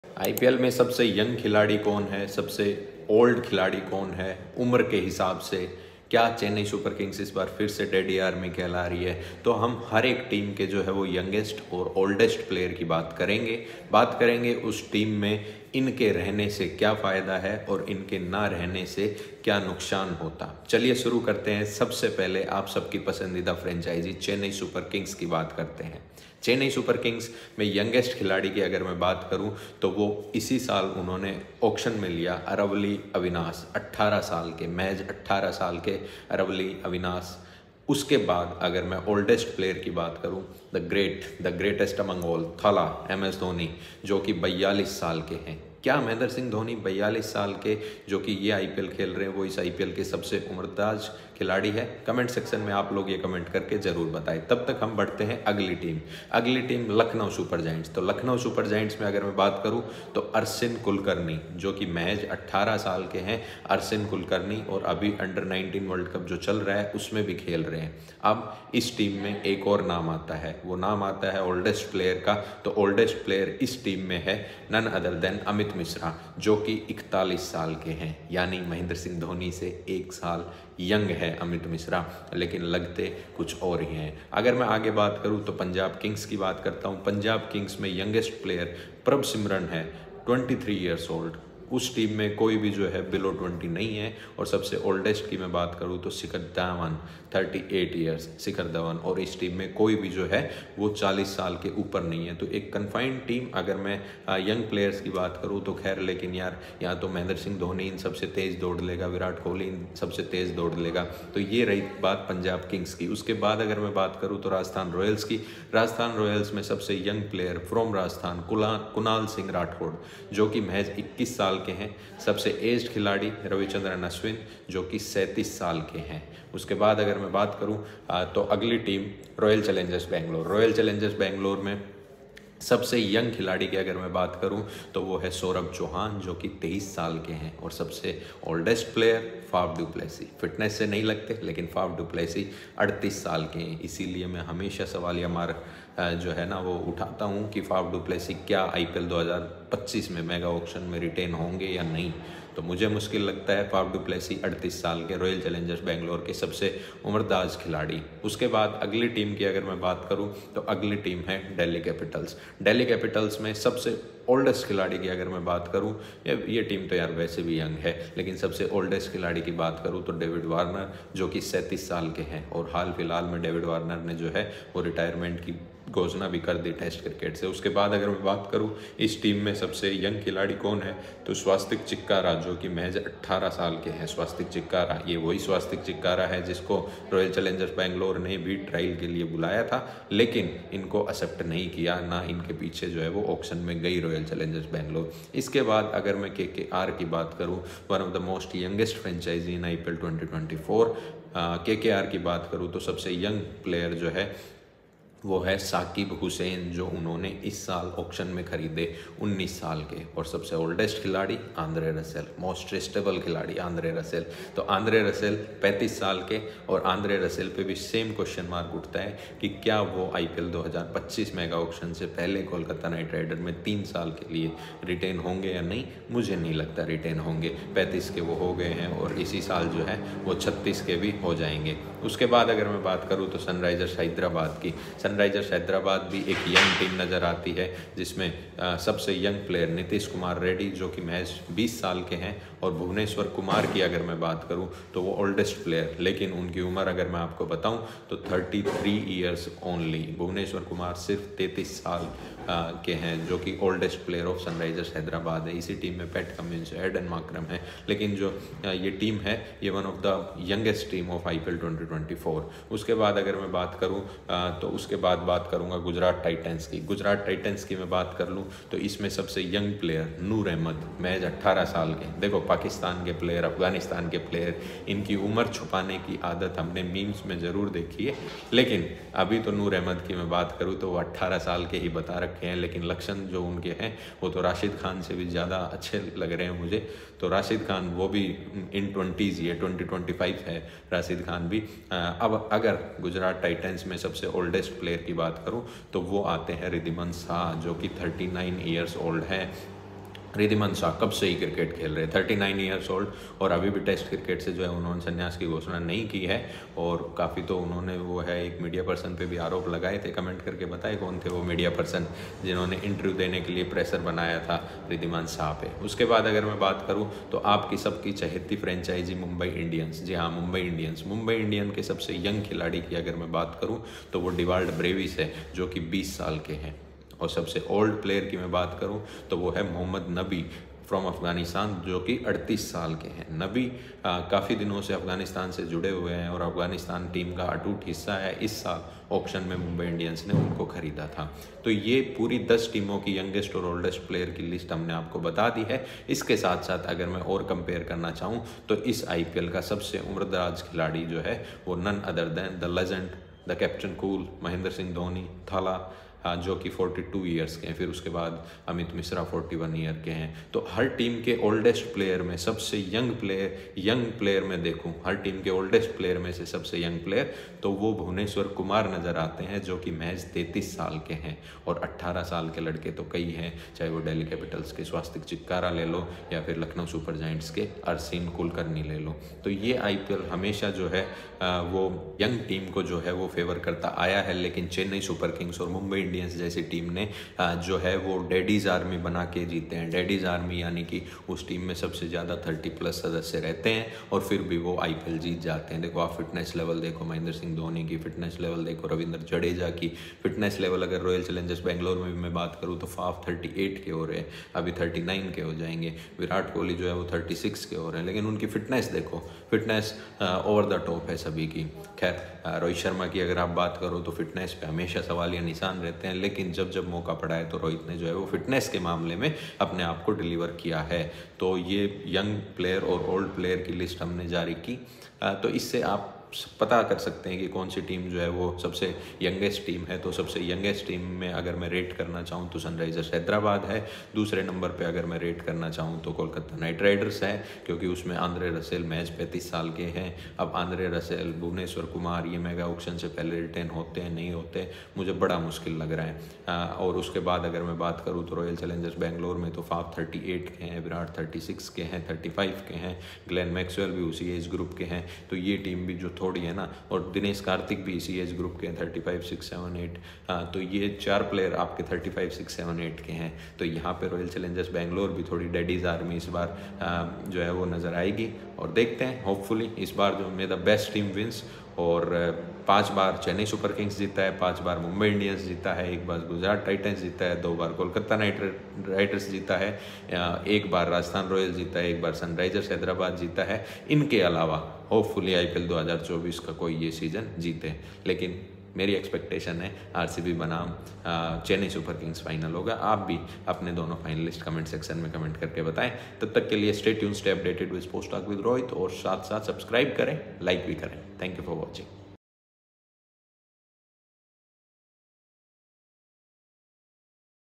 Youngest & Oldest Team & Player of IPL 2024 - MS Dhoni or Virat Kohli or Rohit Sharma?
Youngest and Oldest Team of this IPL 2024! MS Dhoni oldest? No

Hi, This is Rohit Yadav - your brother just a fan like you. Wants to connect as many cricket fans as possible!

Join me live to discuss IPL on our very own Youtube channel - @sportstalkwithrohit